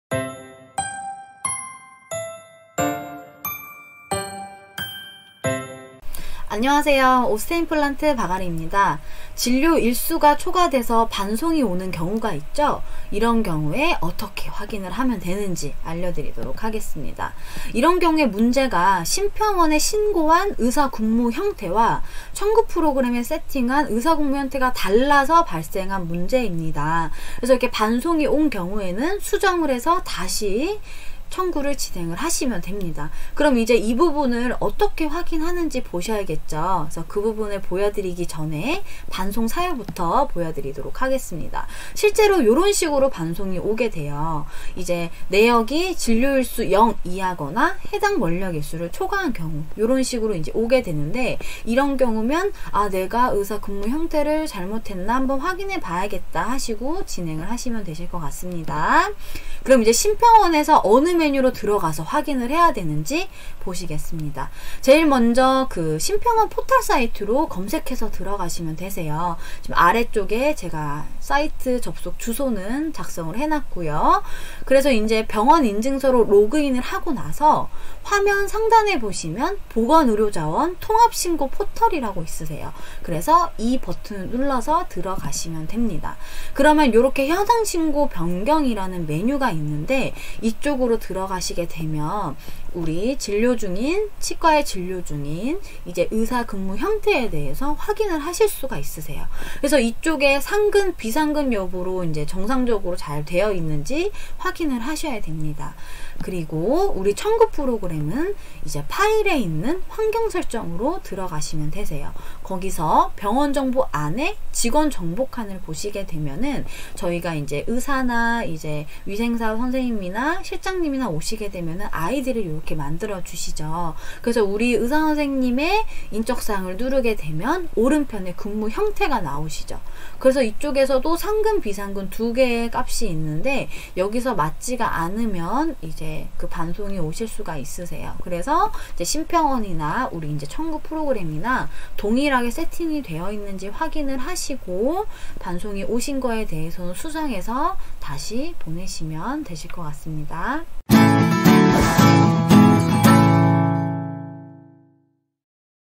[S/W] 진료일수 초과로 반송이 되었어요. 무엇이 문제인가요?
■진료일수 초과로 반송이 된 경우?
- 심평원에 신고한 의사 근무 형태와 청구프로그램의 의사 근무형태가 상이한 경우 발생

■ 심평원 확인 방법
1) 심평원 포탈 사이트 (biz.hira.or.kr) 접속
2) 병원 인증서로 로그인
3) '보건의료자원 통합신고포털' 접속
4) '현황신고/변경' 메뉴에서 의사 근무형태 확인

■ [두번에][하나로] 청구프로그램 확인 방법
1) 파일 - 환경설정
2) 병원정보 - 직원정보 메뉴
3) 해당 의사 선택하여 근무형태 확인
🔎치과 포털 사이트 DENALL 더 알아보기🔍
사이트와 페이스북에서 더욱 다양한 라이브 강연과 치과 콘텐츠를 만나보세요!